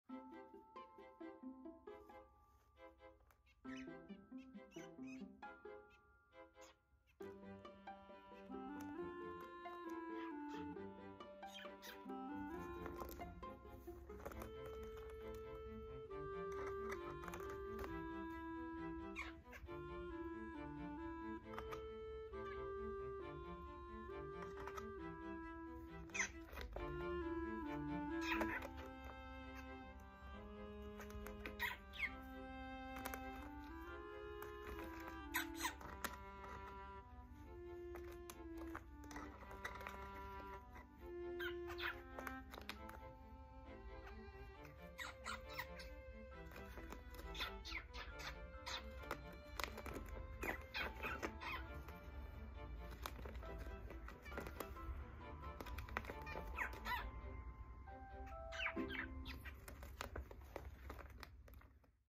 新しい家を見つけたオカメインコと様子を見るキンカチョウ
オカメとチュン太の日常